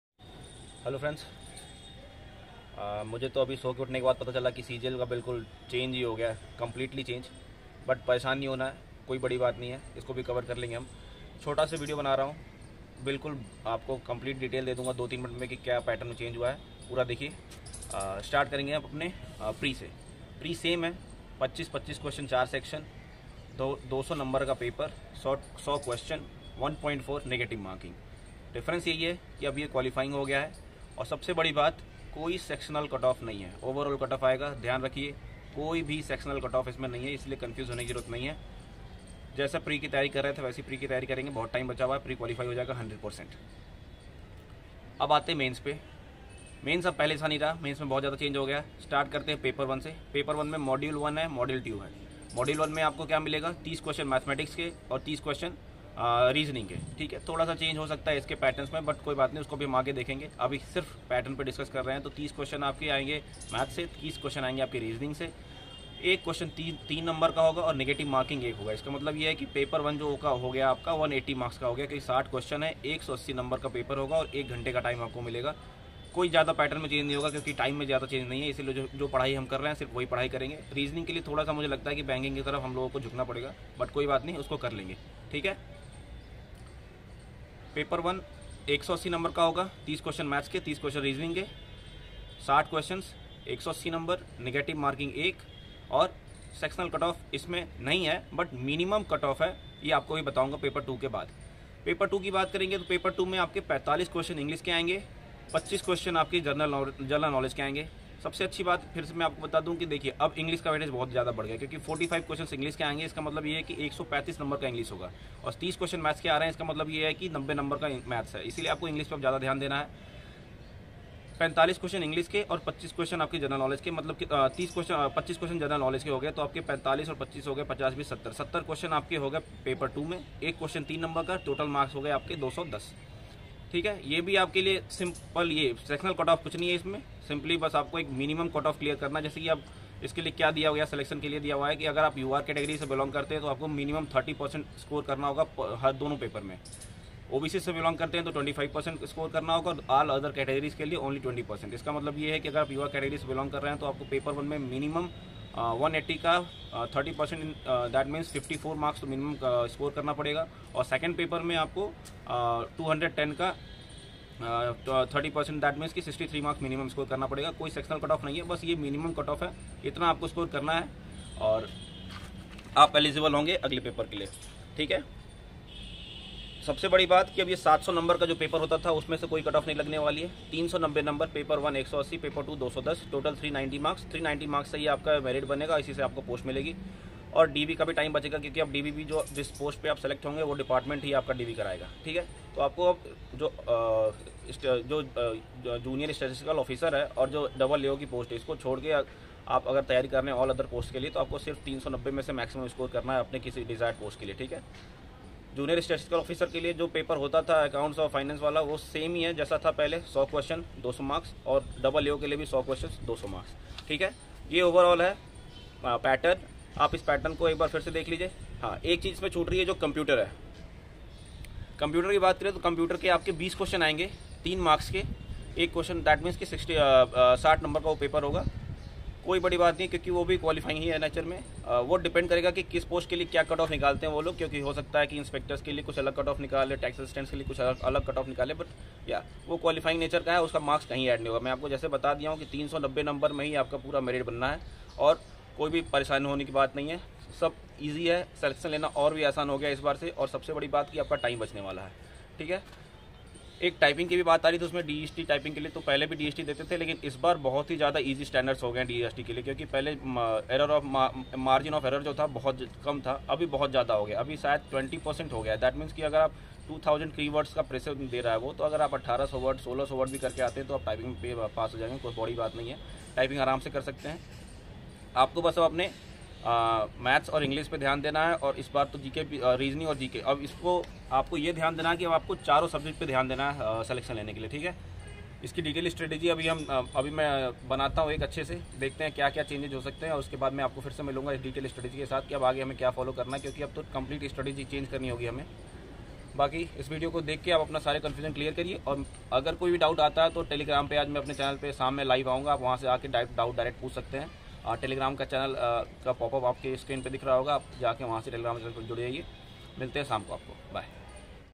CGL 2022 New exam Pattern in detail|CGL 2022 new strategy|Sectional Cutoff Nahi hai?| Passing marks?
All the major changes in cgl 2022 pattern in just 5 mins.
Watch the video and start preparation..
No time left to waste..
Ek ek minute ka use krna hoga...
Jyda time nai hai...
Av naye strategy pe bat krenge..
Next video me..
But tb tk pre ki taiyari krte rho jaise kr rhe ho...
Telegram pe roj live doubts session hota hai..
Search for
CGL Elixr by KK

Inspector Kishan Gupta.